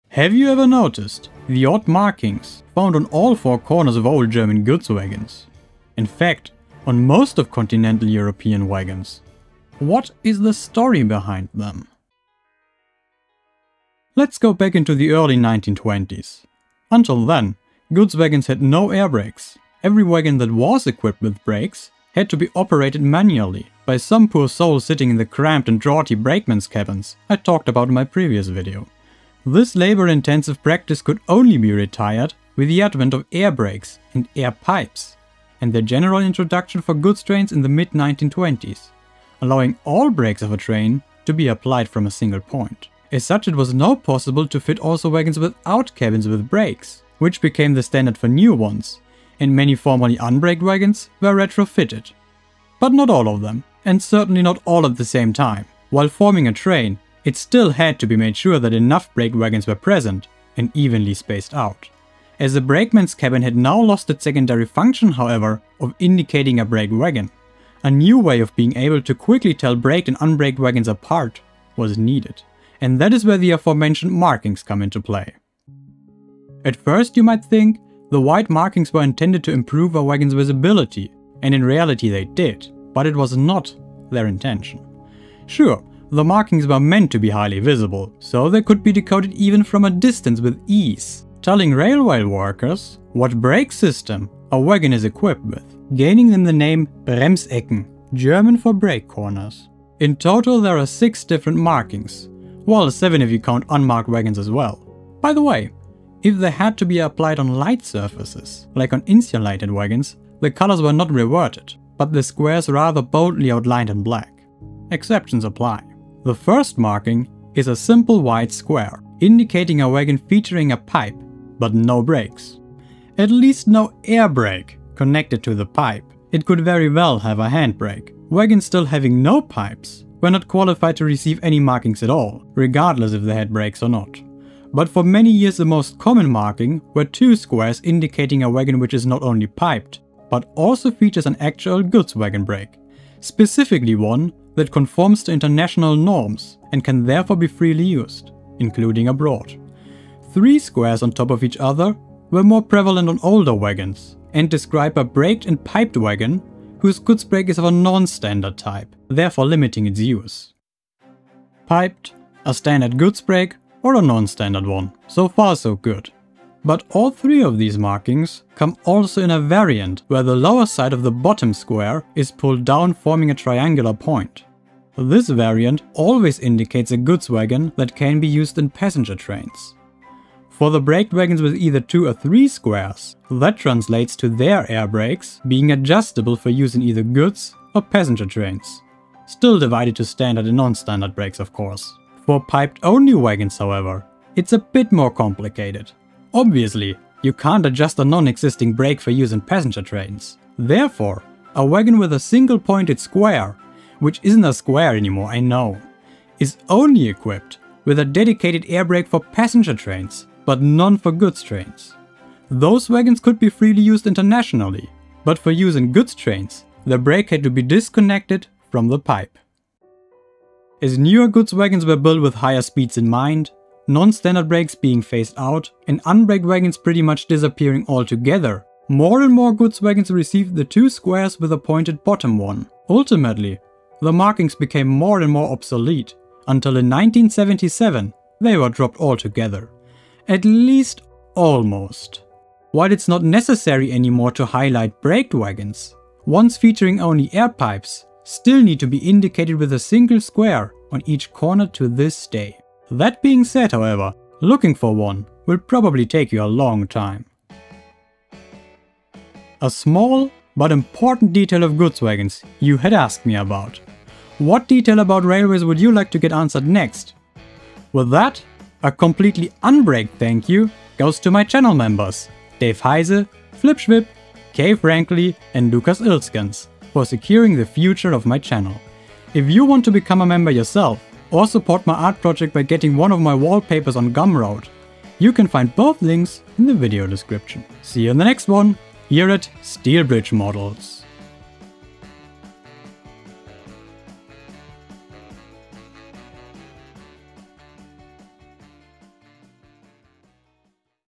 Why Germany painted the Corners of their Wagons.
For decades, most German and continental European goods wagons in general had white squares painted on their corners. What is the reason behind them? Let me explain!

All models in this video were created and rendered by myself using Blender.

Chapters:

0:00 Intro
0:13 Air Brakes
1:16 Painted Corners
3:40 Decline
4:19 Outro